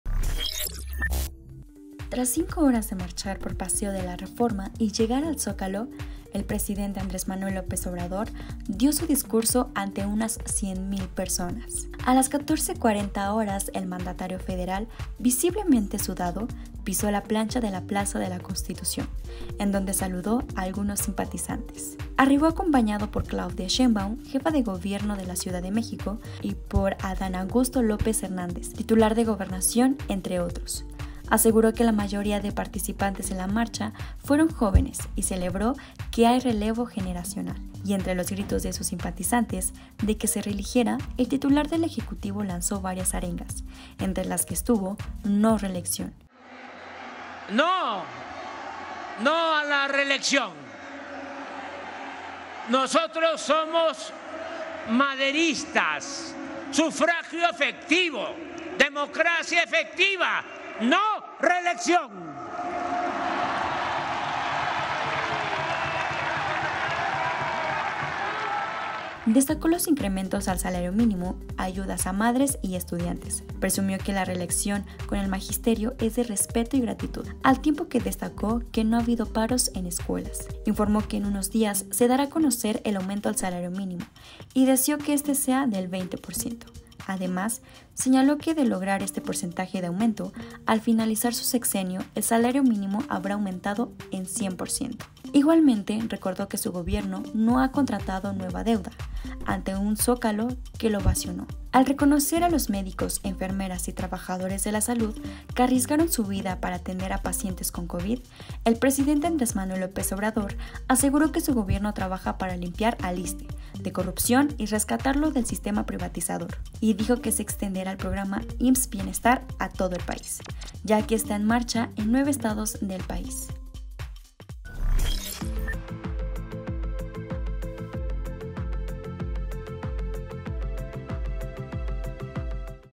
Tras 5 horas de marcha, simpatizantes le piden a AMLO reelección; dice que no
El presidente salió después de las 9 de la mañana del Ángel de la Independencia en una marcha que ha desbordado todo paseo de la Reforma; pide que el aumento al salario mínimo a anunciarse próximamente sea de 20%